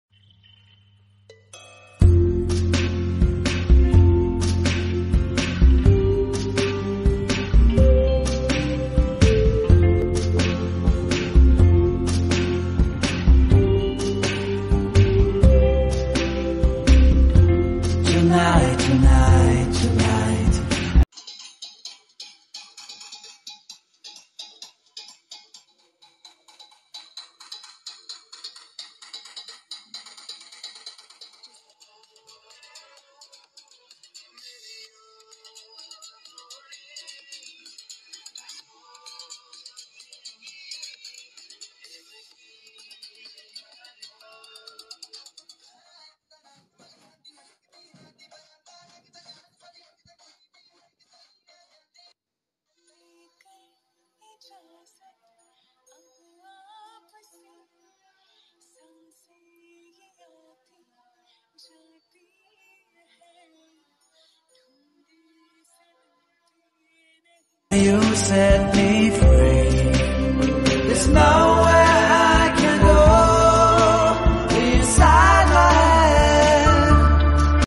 mj ahsan and dr madiha khan marriage | dr madiha khan and mj ahsan Mehndi dance
In this video you can see the mehndi ceremony of Mj Ahsan and Dr Madiha khan.